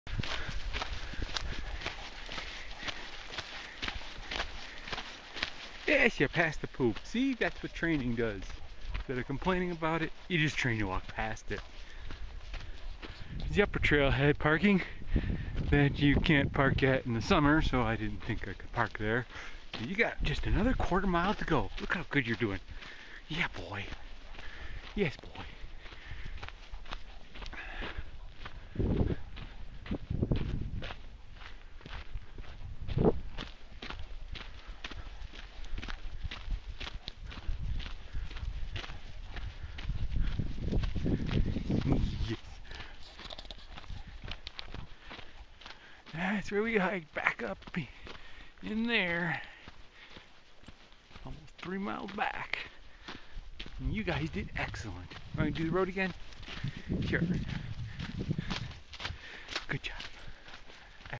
Last and final hike ever....................in 2013
We hiked up to the Old Mill at Staunton State Park. It's a 6 mile round trip and my Buddy Seaman did great. It was a great day for a hike, the person at the gate was so nice that he gave them treats and didn't act like a jerk with the "kepp them on a leash" speach since they are leashed anyway, the ones who don't care won't listen anyway so why tell them for no reason.